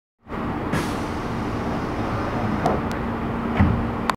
名鉄100系抵抗制御車ドア閉め
125号車にて。
ドアチャイムがないから改造車のような違和感はないです。